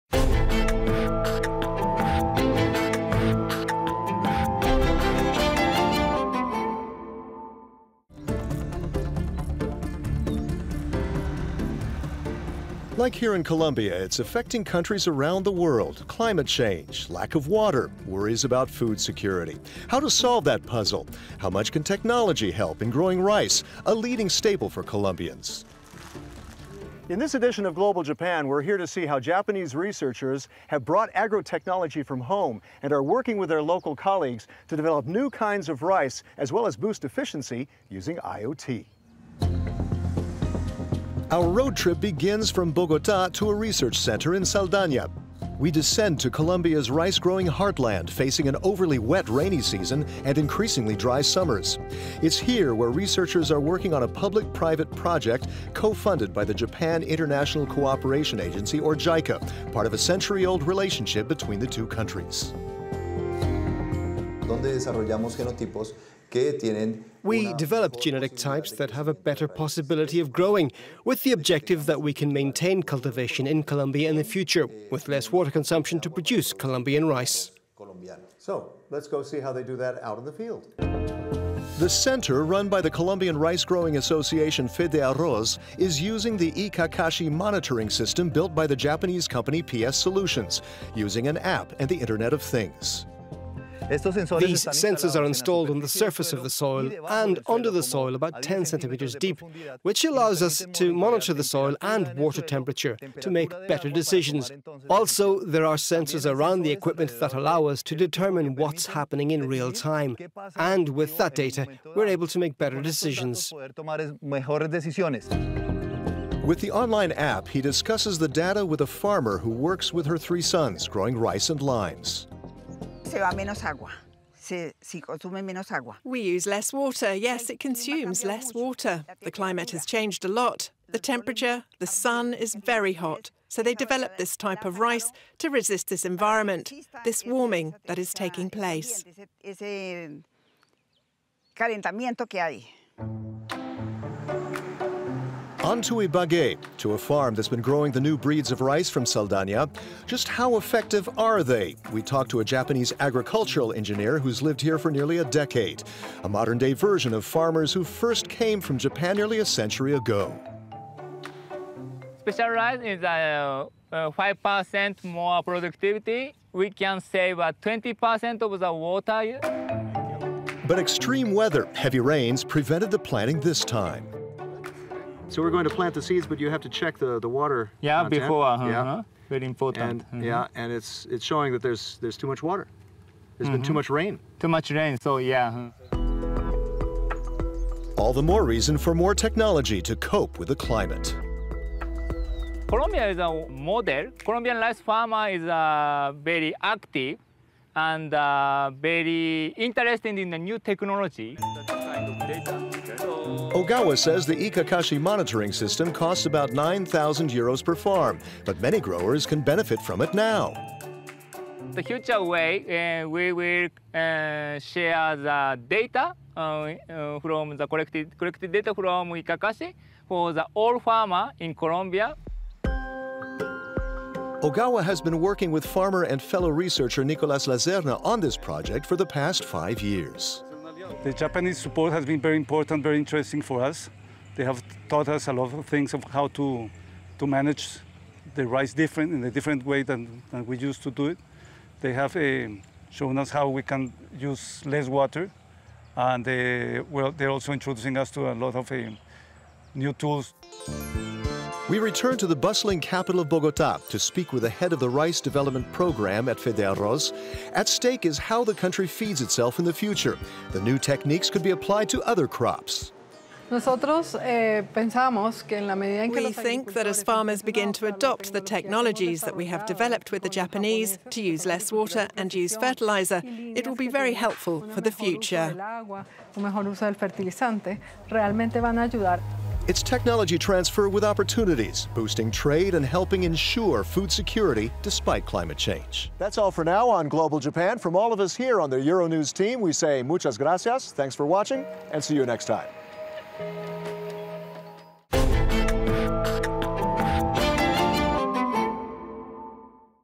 Colombian rice growers fight climate change with Japanese tech
Japanese researchers and a private company help Colombian farmers increase their rice production and use less water, using mobile technology.…